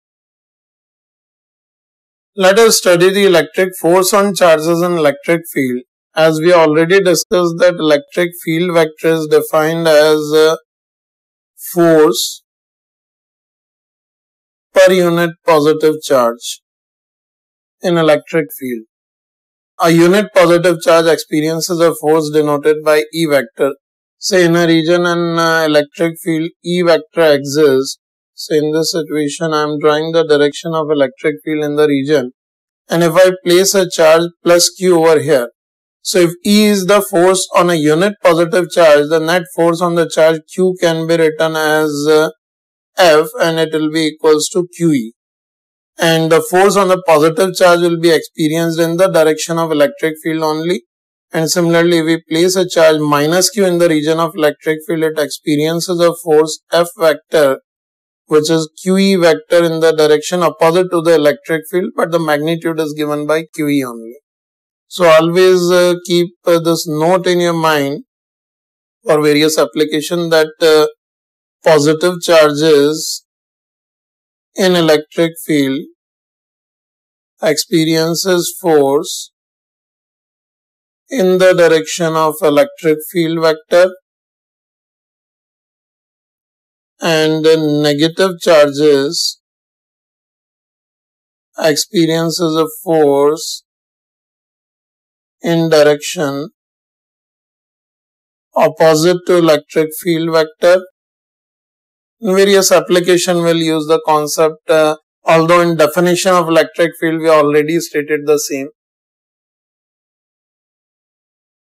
Class 12 Physics | Electrostatics | #21 Electric Force on Charges in Electric Field | For JEE & NEET
PG Concept Video | Electrostatics | Electric Force on Charges in Electric Field by Ashish Arora

Students can watch all concept videos of class 12 Electric force & electric field for jee & neet in proper sequence through PG channel playlist.